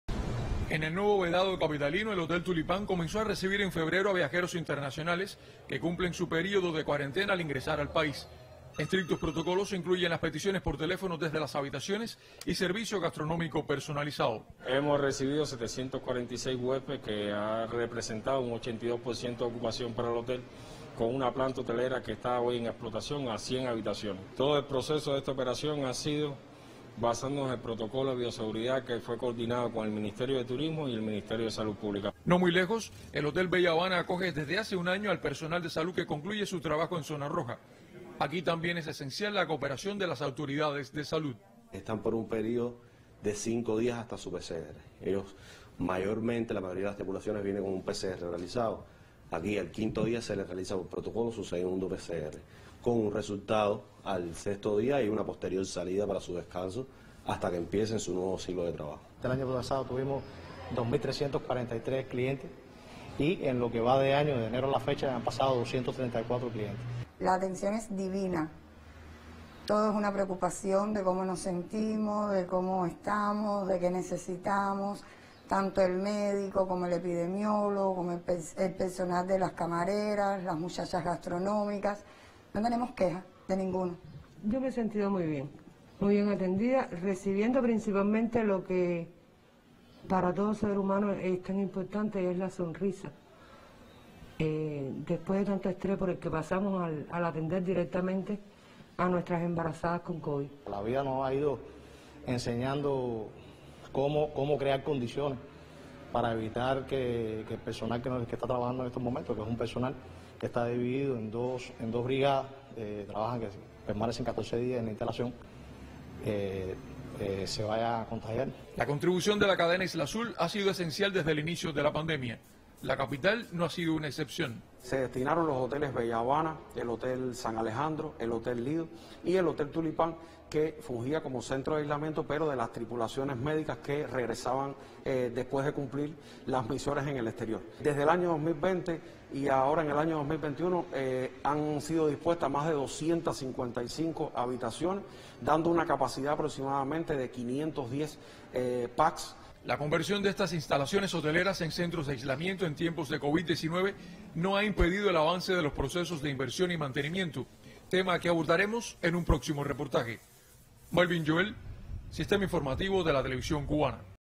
Cadena Islazul de Cuba en tiempos de pandemia
Decenas de instalaciones hoteleras en toda Cuba se han sumado a la estrategia de enfrentamiento a la COVID19. En ese escenario, la cadena Islazul dispone en la capital de más de 250 habitaciones para el aislamiento del personal de salud y viajeros internacionales.